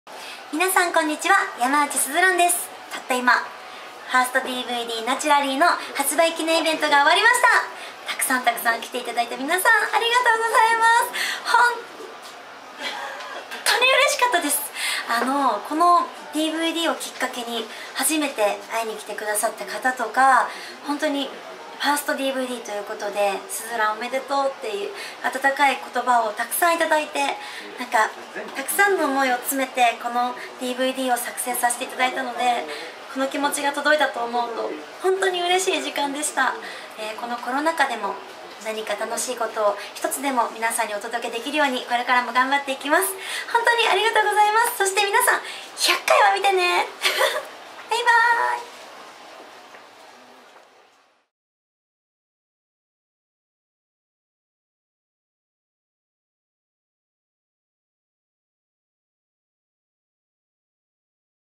【山内鈴蘭】1stDVD発売記念イベント・終了後コメント@HMV♪
■山内鈴蘭「Natural Lily」
SKE48・山内鈴蘭の1stイメージDVD！

ファン待望の1stDVDが遂に登場。
ドラマ・バラエティ・ライブステージとは違った山内鈴蘭の魅力満載！
元気＆セクシー、最高の1作がここにある！

出演者：山内鈴蘭